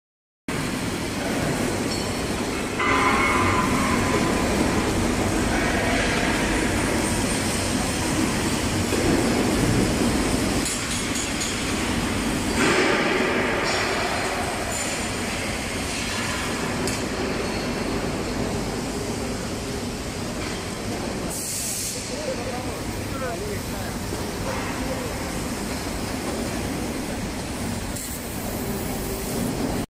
ACME 3 Chuck 0 Tailing Automatic Loading CNC Tube Laser Cutting Machine
【ACEM Laser】Real-time factory exploration by laser pipe cutting machine.
△Independently developed chuck and bed
△Extra-long tube customization service
△More excellent performance, better stability
We are still constantly developing, innovating and improving!
If you have any questions, look forward to hearing from you~